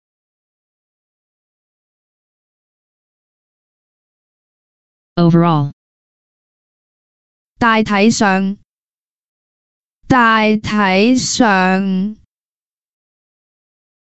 "OVERALL" in Cantonese (大體上) - Flashcard
[daai6 tai2 seung6] "OVERALL" in Cantonese (大體上) - Flashcard. Additional information about the word: overall,in general terms.